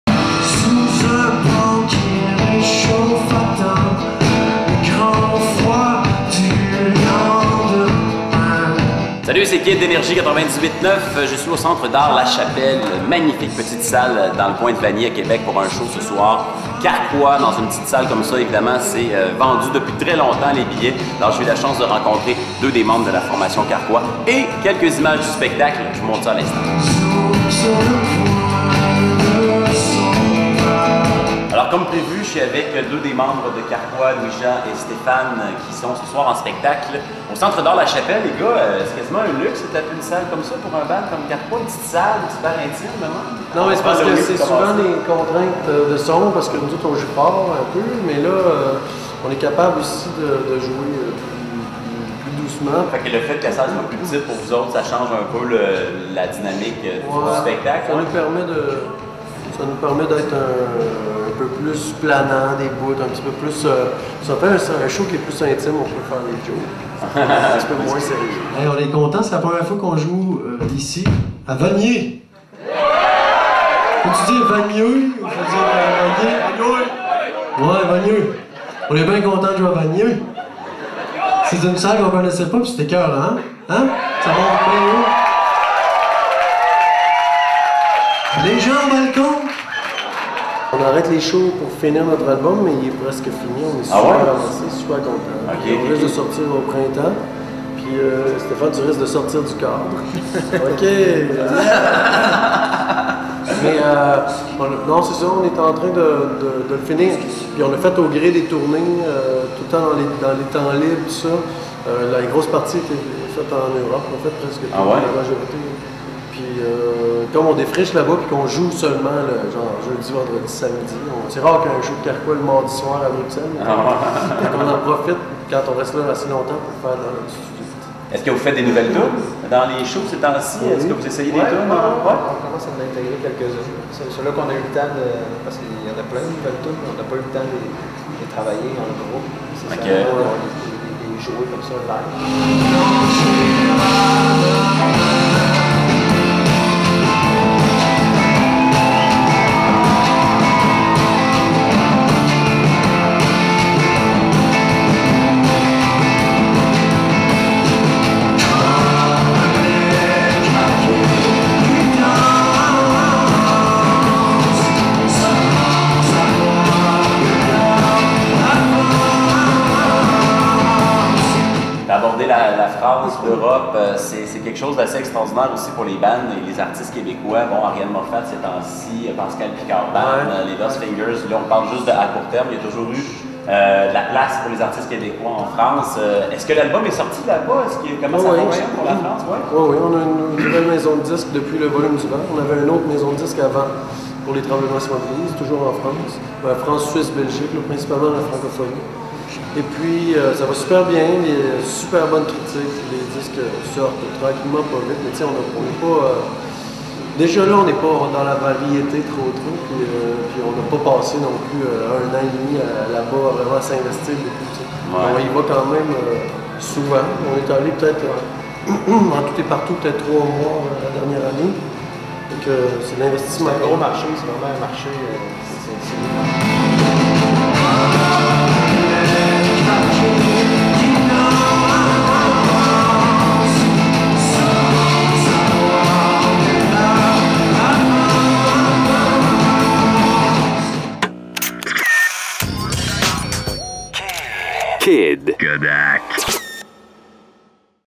Kid d'NRJ 989 rencontre Karkwa en spectacle à Québec
Le groupe Karkwa était de passage au centre d'art la Chapelle de Québec le 28 novembre dernier. Phil "Kid" Jacques d'NRJ 98.9 en a profité pour parler avec eux d'un nouvel album à venir.